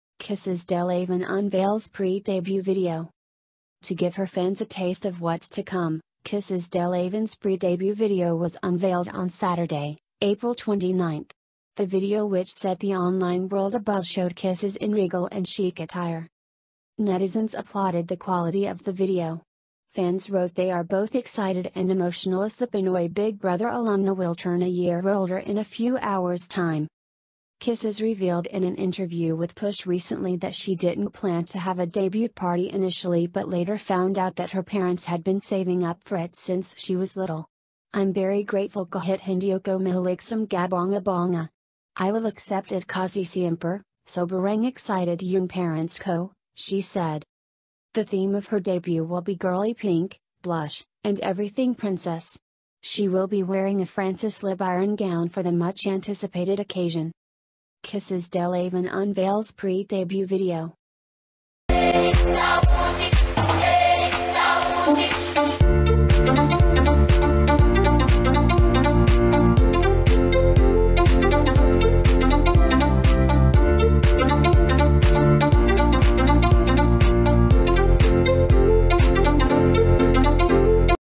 Kisses Delavin unveils pre-debut video
Philippines News today : Entertaiment scandals

To give her fans a taste of what’s to come, Kisses Delavin’s pre-debut video was unveiled on Saturday, April 29. The video which set the online world abuzz showed Kisses in regal and chic attire.

Netizens applauded the quality of the video. Fans wrote they are both excited and emotional as the Pinoy Big Brother alumna will turn a year older in a few hours time.

Kisses revealed in an interview with PUSH recently that she didn’t plan to have a debut party initially but later found out that her parents had been saving up for it since she was little. “I’m very grateful kahit hindi ako mahilig sa mga bongga-bongga. I will accept it kasi syempre, sobrang excited yung parents ko,” she said.

The theme of her debut will be “girly-pink, blush, and everything princess.” She will be wearing a Francis Libiran gown for the much anticipated occasion.